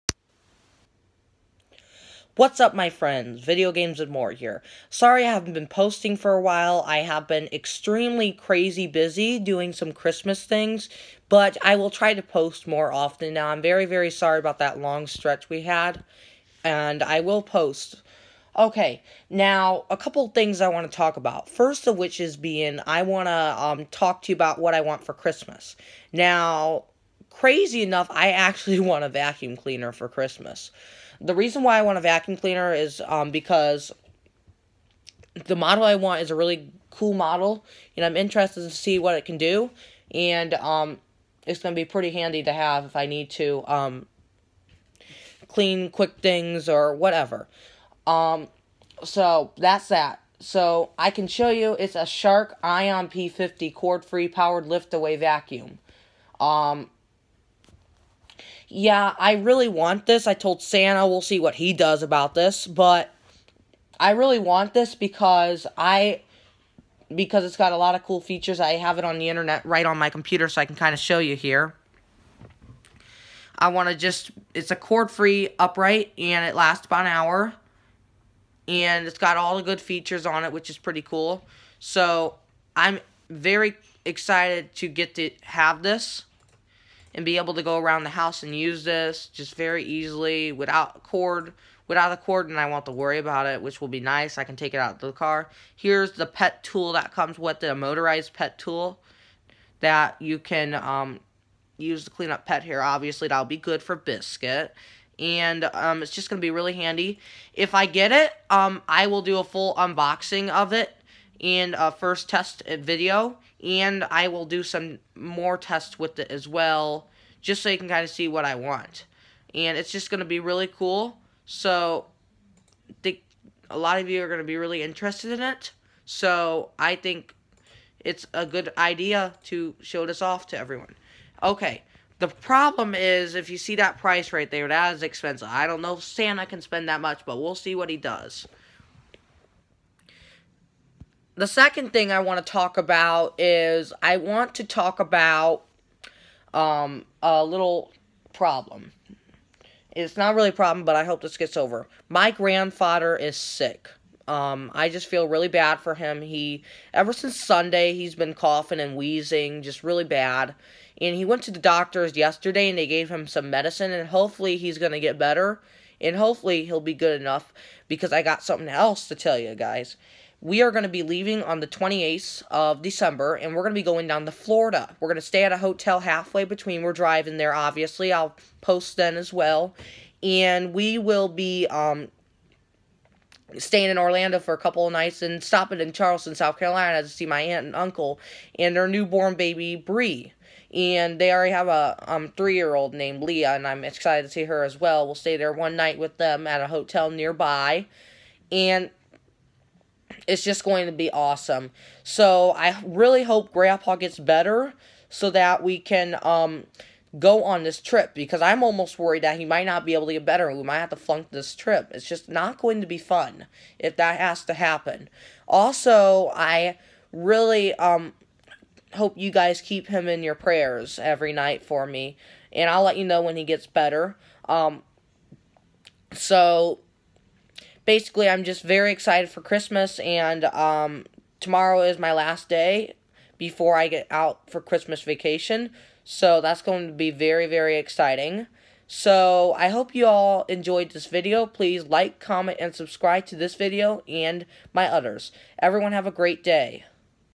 What I want for Christmas
This video describes what I want for Christmas and about my upcoming trip to Florida.